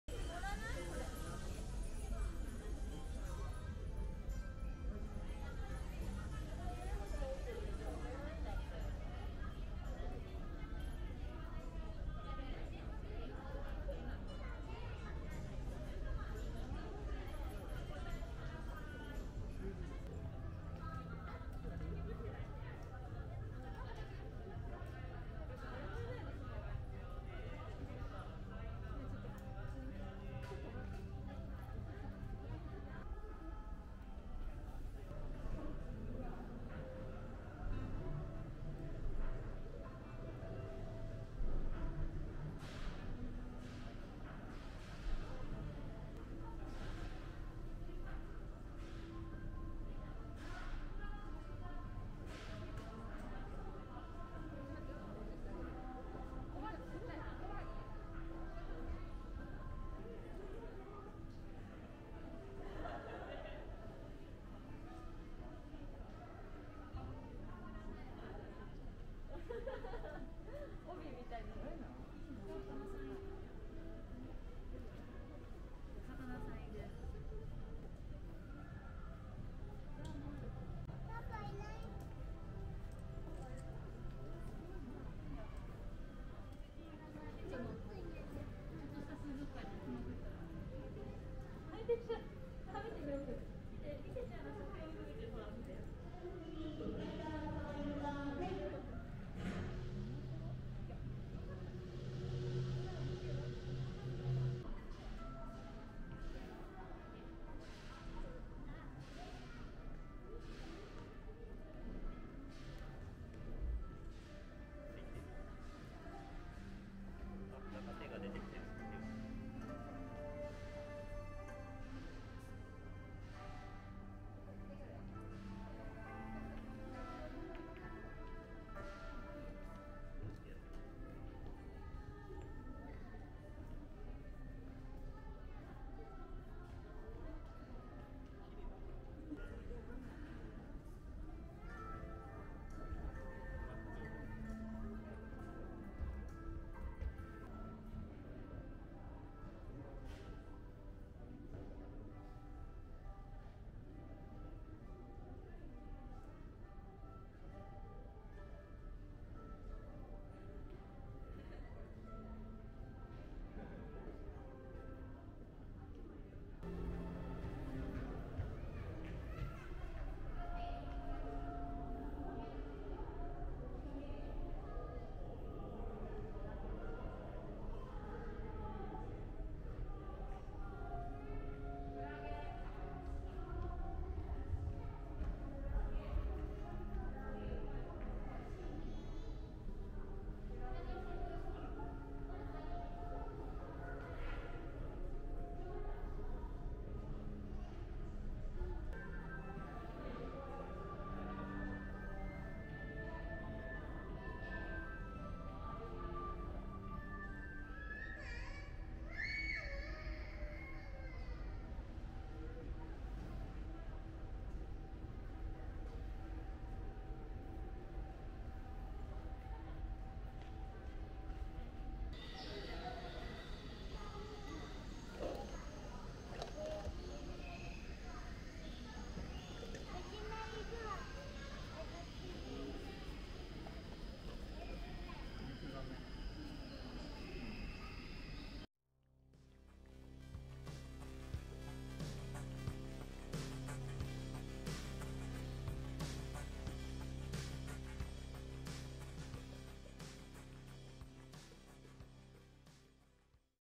【カピ活】桜舞う水族館(マクセルアクアパーク品川/maxell AQUA PARK SHINAGAWA)
春のアクアパーク品川は桜で満開。
NAKED SAKURA AQUARIUM(2022年3月12日～4月17日)

【場所/place】
maxellアクアパーク品川(maxell AQUA PARK SHINAGAWA)